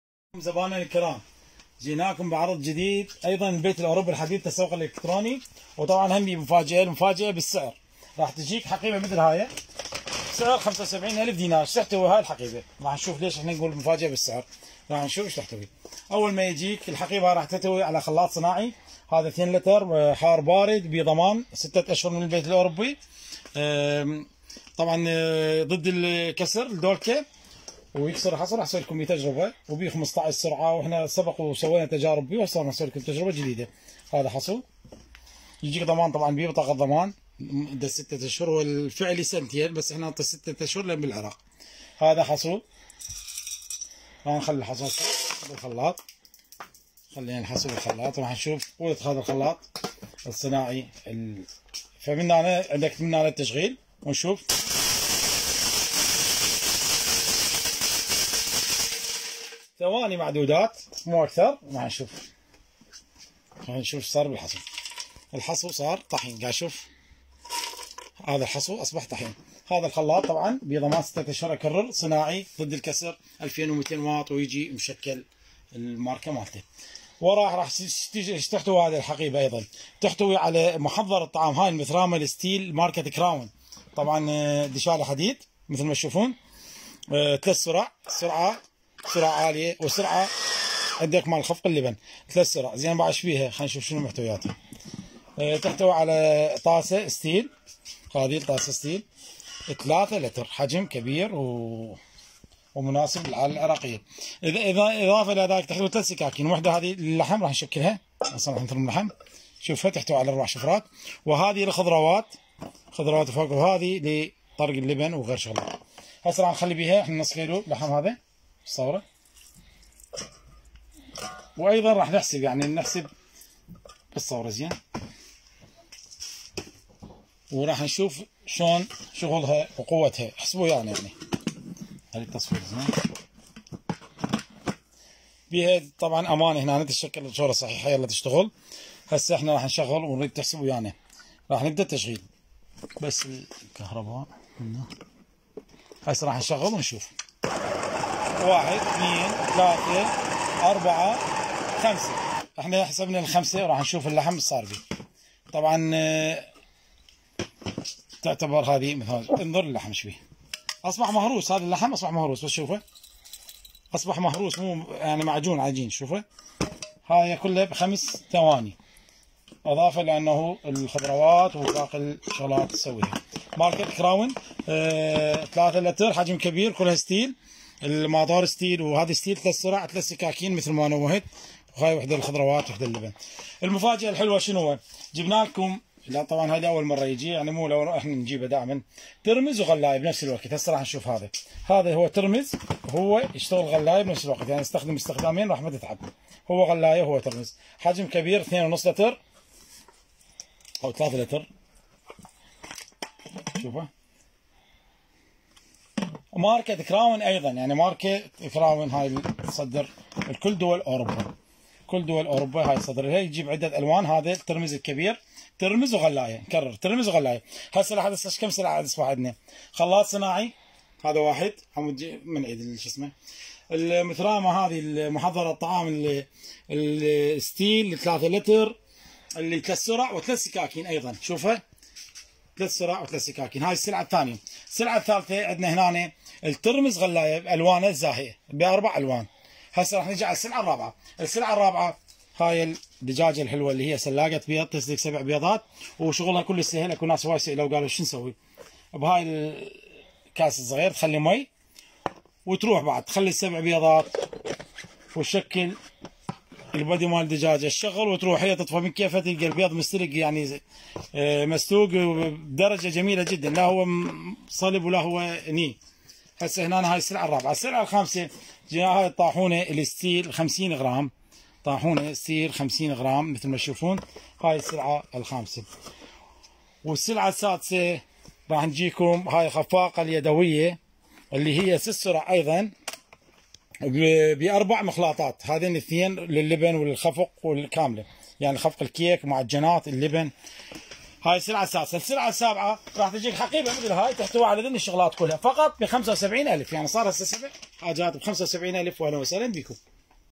عرض مفاجأة لعام 2020 أجهزة كهربائية منزلية عدد ٦ بسعر ٧٥ ألف فقط
عرض مفاجأة لعام 2020
٦ أجهزة كهربائية منزلية بسعر ٧٥ ألف فقط
1- خلاط صناعي حجم كبير ٢ لتر ضد الكسر دشالي حديد وارد الماني
2-محضرة طعام المثرامة الستيل حجم ٣ لتر ٣ سرع ٣ سكاكين تستخدم لثرم اللحم والخضروات ماركة كراون
3-ترمز غلاية كهربائي حجم كبير ٢ونصف لتر
 ماركة كراون
4-سلاقة بيض على شكل دجاجة تسلق ٧ بيضات
5-طاحونة كهربائية ستيل ٥٠ غرام
6-خفاقة يدوية كهربائية تحتوي على أربع مخلاطات ستيل
للحجز والاستفسار مراسلتنا على الصفحة أو الإتصال عبر الأرقام
توجد خدمة توصيل بغداد ٥ ألف
المحافظات ١٠ ألف
سيتم اضافة هدية عند شراء اكثر من طلب
سيتم إشراك الزبائن بقرعة الجوائز
عنواننا زيونة مجاور مول زيونة
بغداد الجديدة مقابل افران الدرويش